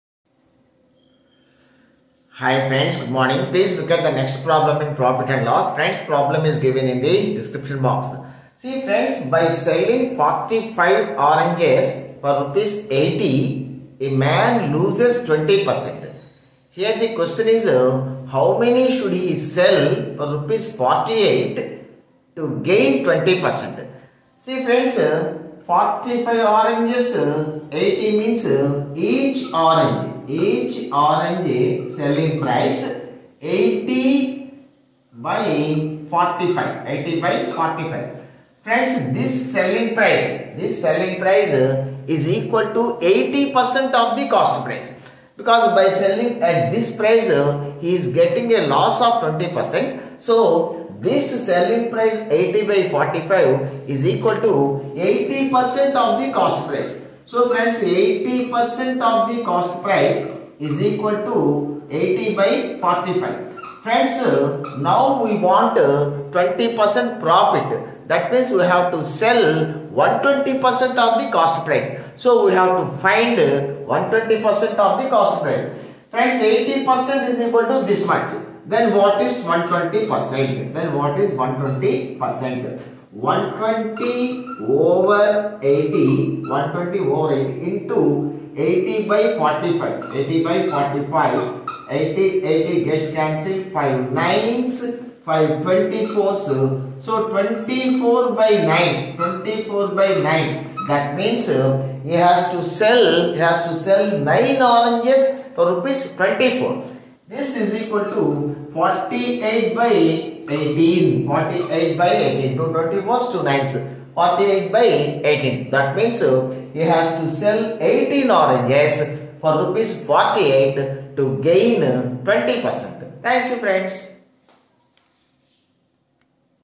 By selling 45 oranges for ₹80, a man loses 20%. How many should he sell for ₹48 so as to gain 2
By selling 45 oranges for ₹80, a man loses 20%. How many should he sell for ₹48 so as to gain 20% in this transaction?